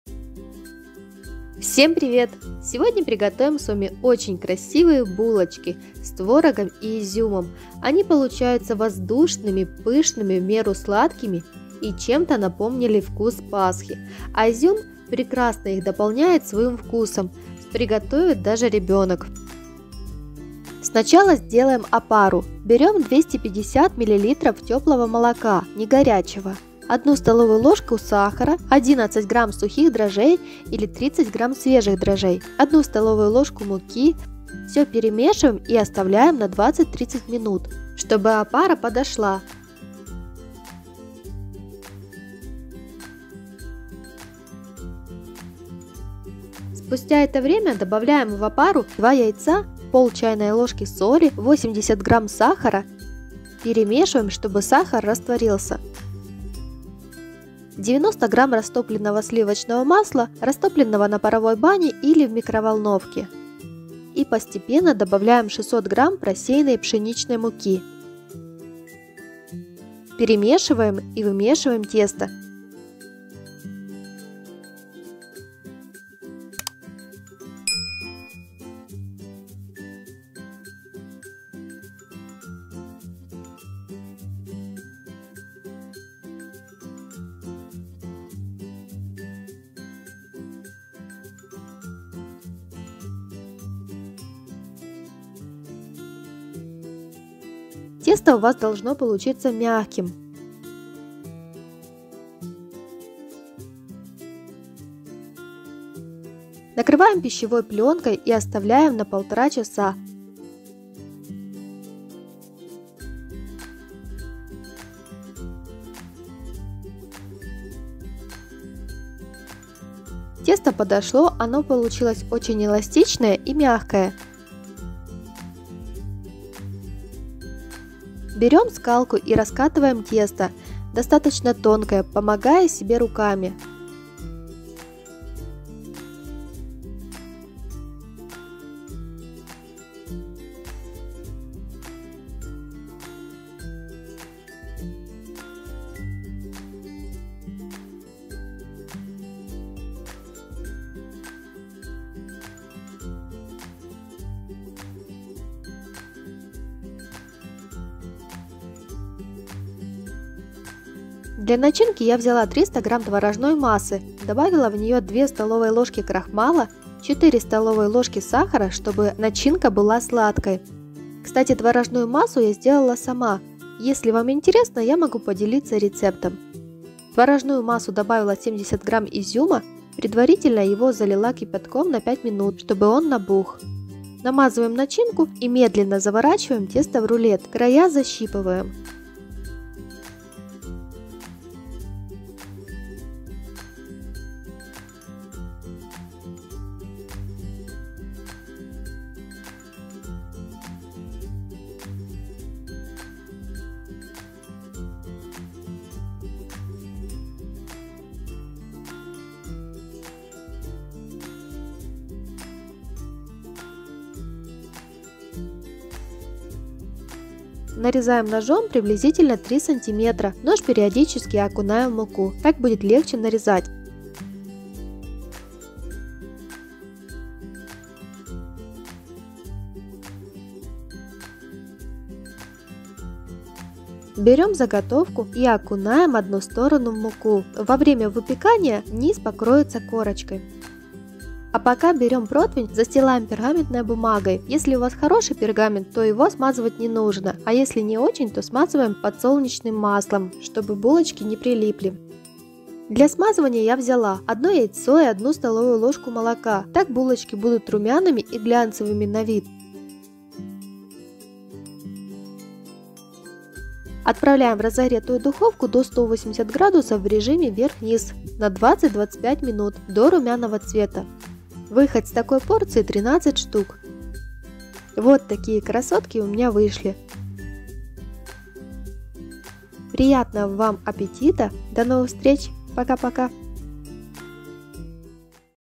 Булочки с творогом и изюмом.
Готовим булочки с творожной начинкой и изюмом, выглядят очень красиво и аппетитно, готовятся просто, а получаются очень вкусно.
    Приятного аппетита! До новых встреч!

Рецепт:

Опара:
250 мл. молоко
1 ст. л. сахар
1 ст. л. мука пшеничная
11 гр. дрожжи сухие или 30 гр. свежих дрожжей

Тесто:
Готовая опара
2 шт. яйцо (130 гр.)
0,5 ч. л. соль
90 гр. сливочное масло
80 гр. сахар
600 гр. мука пшеничная

Начинка:
300 гр. творожная масса
4 ст. л. сахар
70 гр. изюм
2 ст. л. крахмал

Смазка:
1 шт. яйцо
1 ст. л. молоко

Выход  13 штук

Разогретая духовка до 180*С 20-25 мин. в режиме верх-низ до румяного цвета.